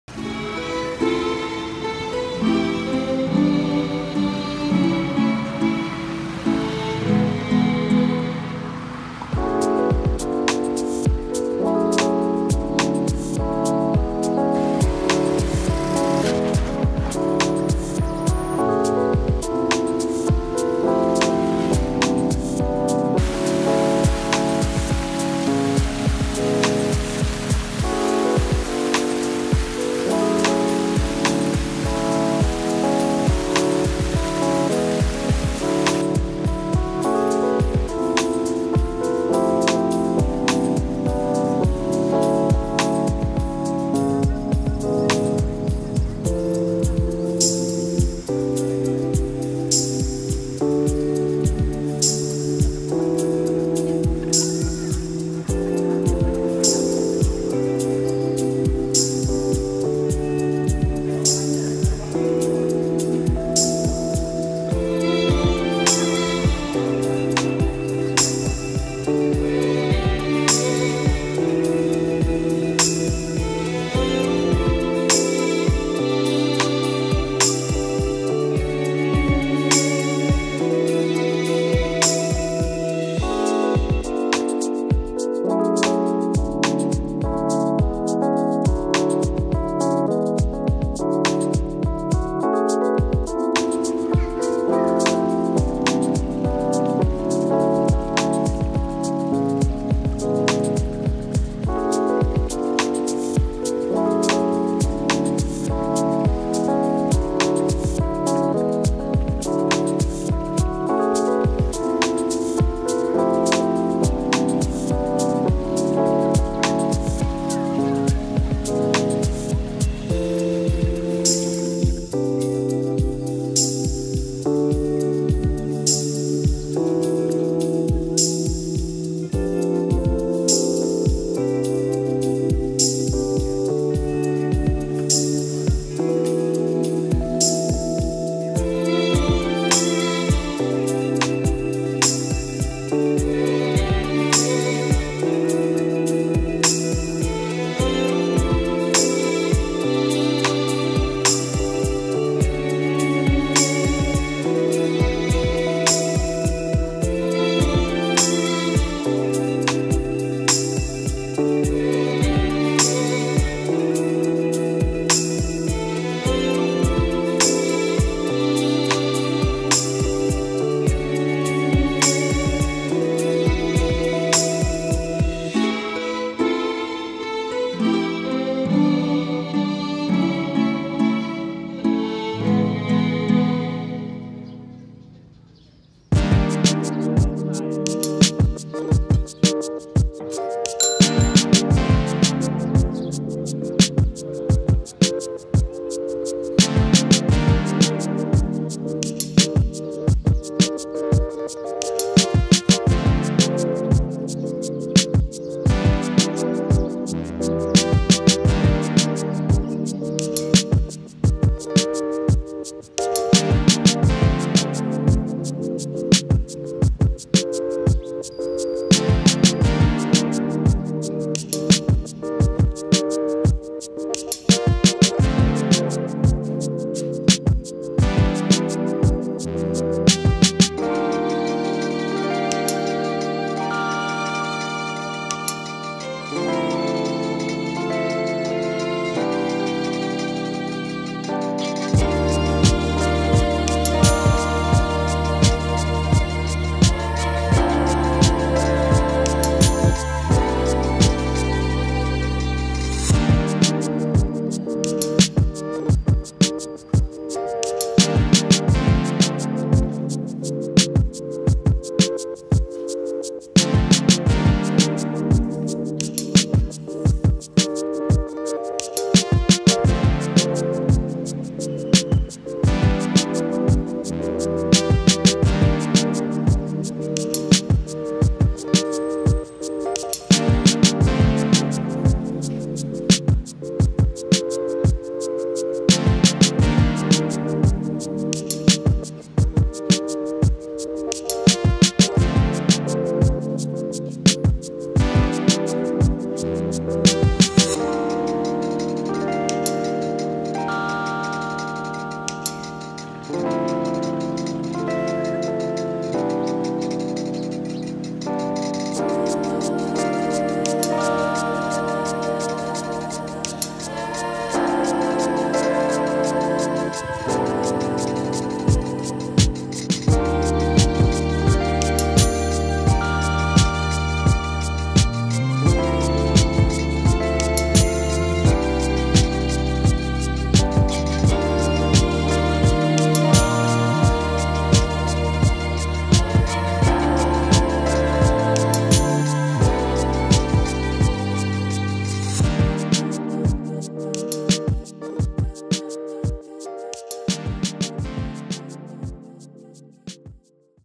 Vientiane (Laos) : tourist guide in english - guide tour about this destination 🇱🇦
Vientiane  and the surrounding area in Vientiane  (Laos) : visitation of the Vientiane  in video 4K. Video tour guide of Vientiane . Visit Vientiane . Plan your next trip to Vientiane  by viewing the top 10 tourist attractions in Vientiane . For your holidays in Vientiane , discover the list of tourist attractions of Vientiane  with the Gay Traveler (Gay Voyageur). Gay Vientiane .

Visit of Vientiane :
Vientiane : @00:01
Independance Monument: @00:14
Crowne Plaza Vientiane : @01:19
Pha That Luang : @02:09
Wat Sisakhet : @03:37
Gay guide of Vientiane : @05:58

Animation: Danny Kronström
Montage: Martin Kronström
Camera: Danny Kronström